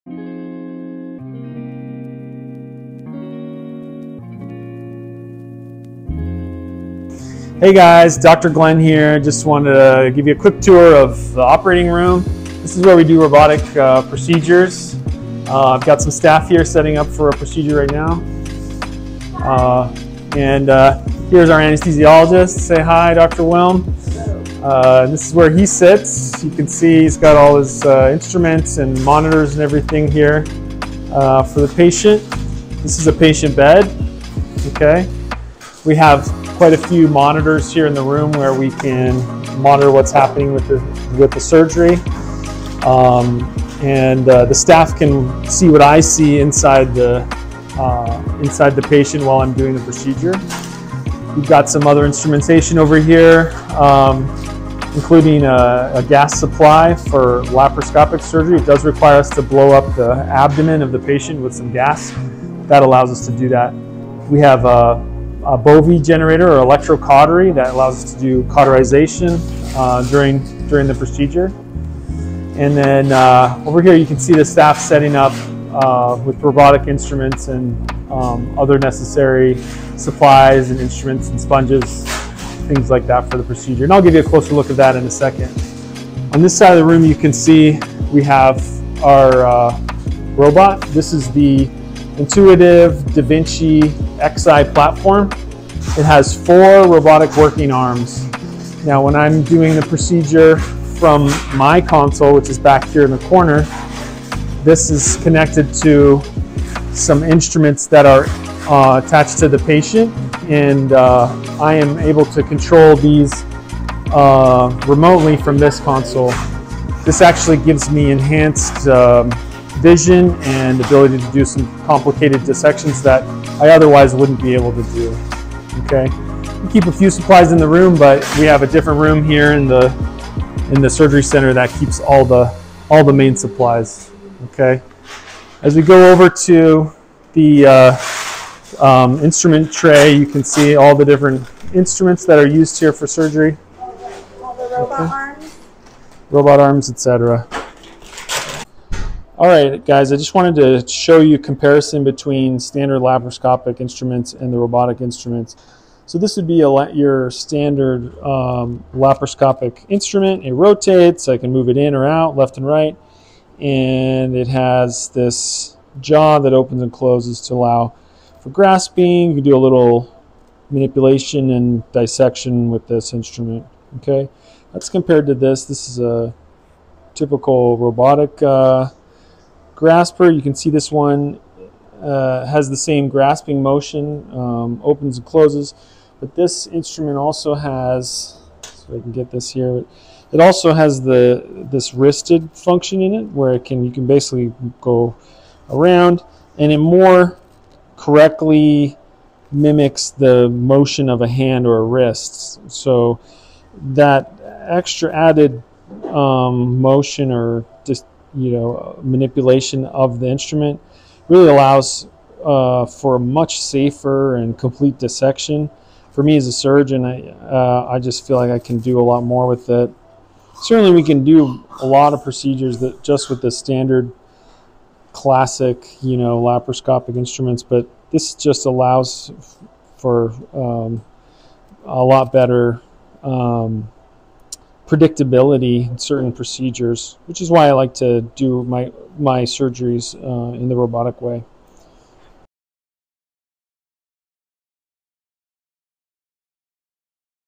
Robotic OR tour
Robotic operating room tour by Dr. Jordan Glenn DO FACS. Dr Glenn is a general surgeon in the Phoenix area.
Check us out :
Glenn-surgery.com